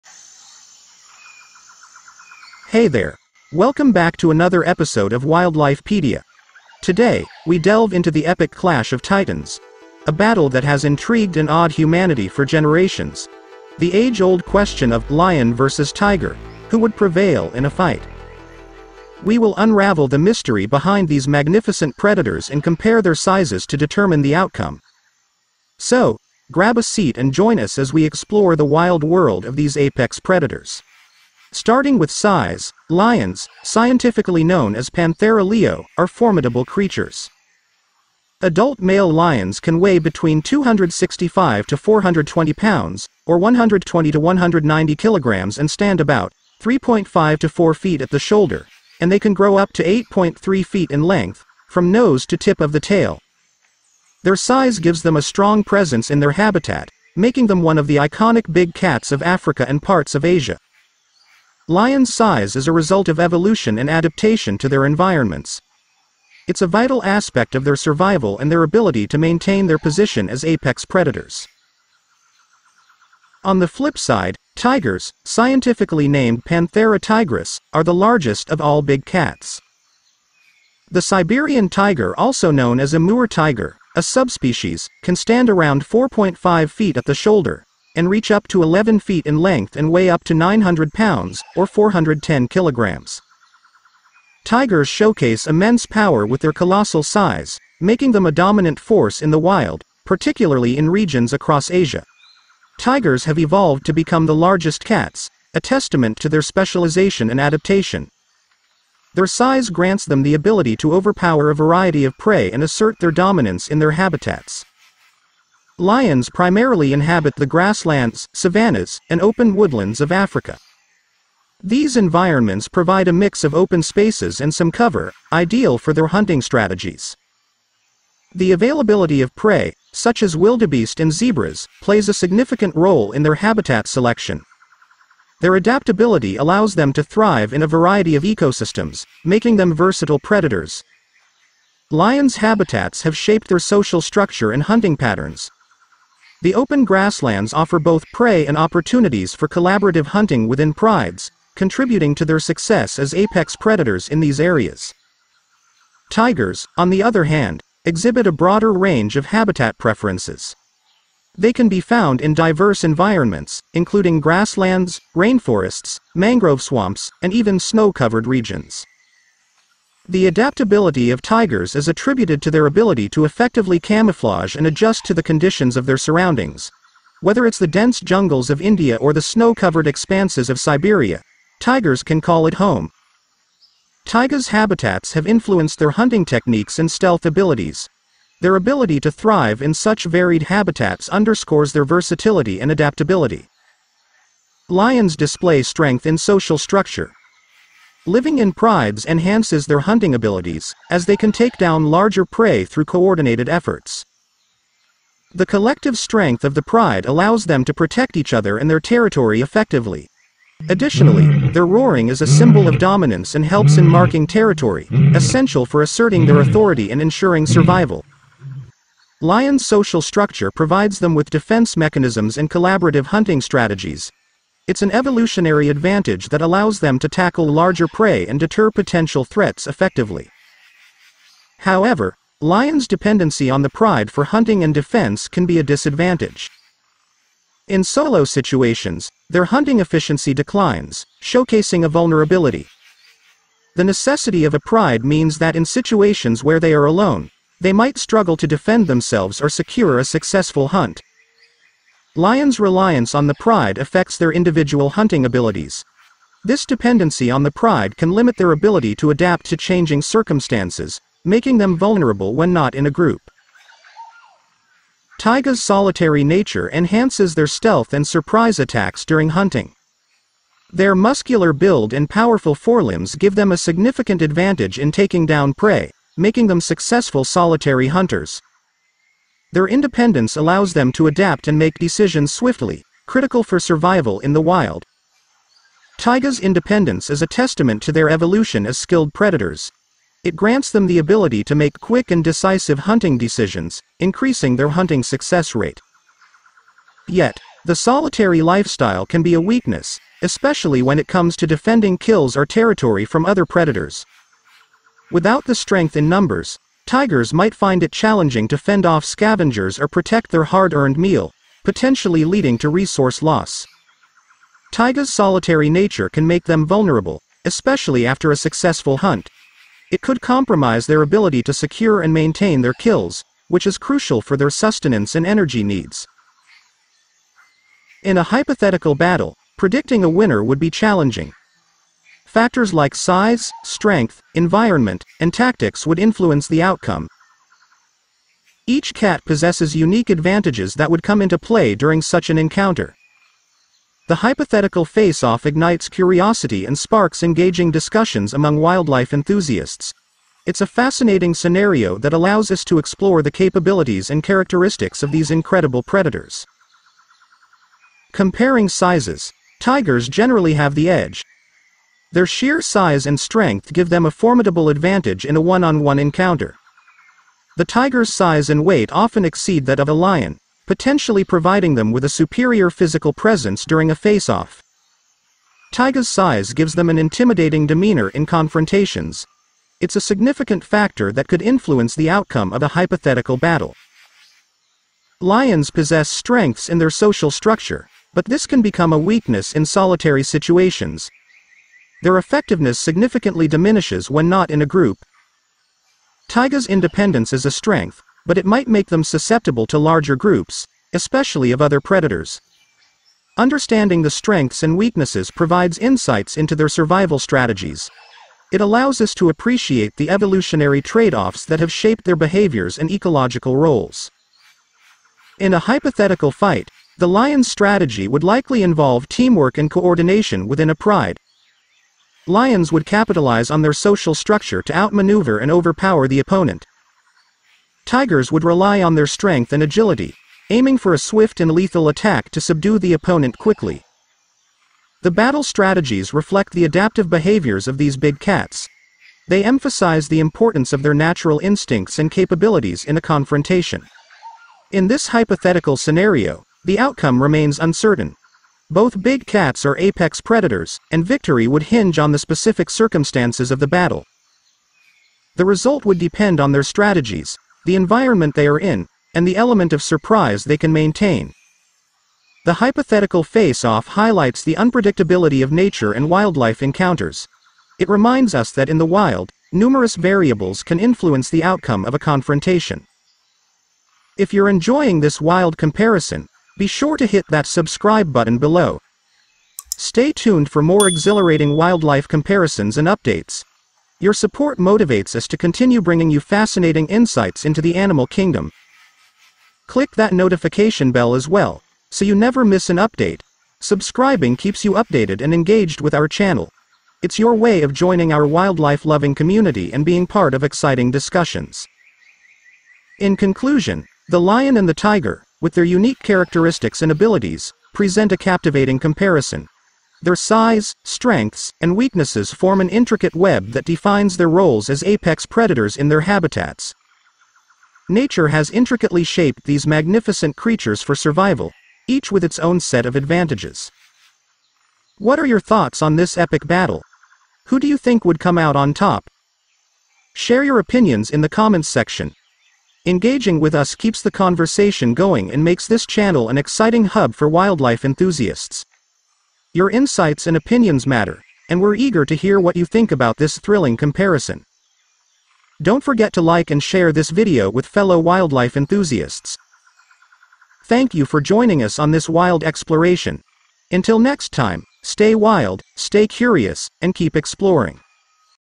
Lion vs Tiger: Who Is More Powerful | Speed and Agility Face-Off
Welcome to the epic battle and detailed comparison between the lion and the tiger, the big cats ruling the wild. Explore their unique attributes, strengths, and fierce fighting techniques that set them apart. Join us in this visual journey to witness their might and make an informed choice on who would triumph in a face-off.